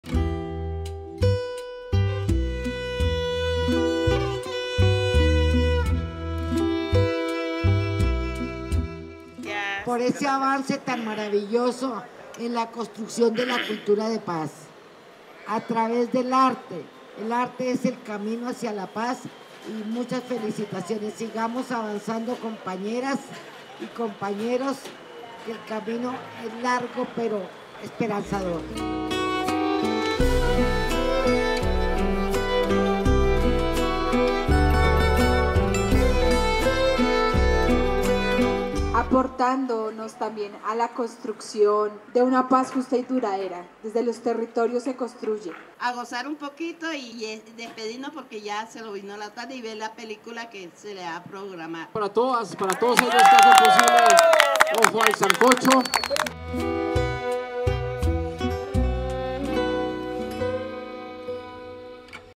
Día 8 estrategia ojoalsancocho en el barrio Perdomo Alto, Ciudad Bolívar, Bogotá diciembre 2023
Invitados: Fundación Fundicar

Organiza: Sueños Films Colombia,
Apoyan: Ministerio de las culturas, las artes y los saberes.
Fondo mixto de cultura Nariño.